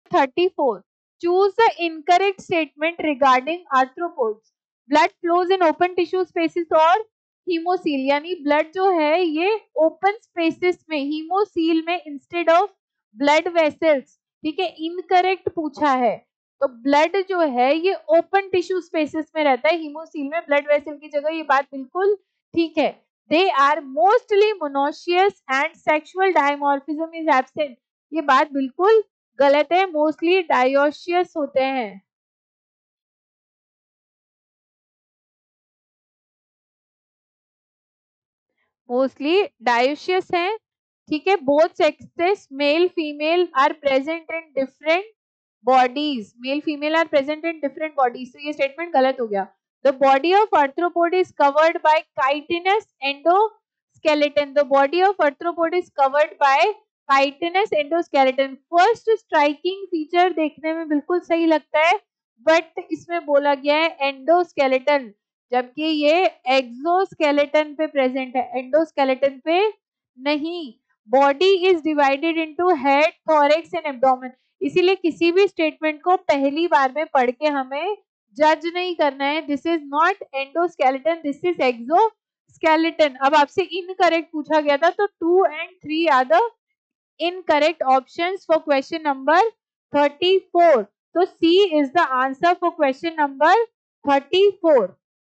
Choose the incorrect statements regarding arthropods- (1)&nbsp;&nbsp; Blood flows in open tissue....
Choose the incorrect statements regarding arthropods-
(1)&nbsp;&nbsp; Blood flows in open tissue spaces or hoemocoel instead of blood vessels.
(2)&nbsp;&nbsp; They are mostly monoecious and sexual dimorphism is absent.
(3)&nbsp;&nbsp; The body of Arthropods is covered by Chitinous endoskeleton
(4)&nbsp;&nbsp; Body is divided into head, thorax and abdomen.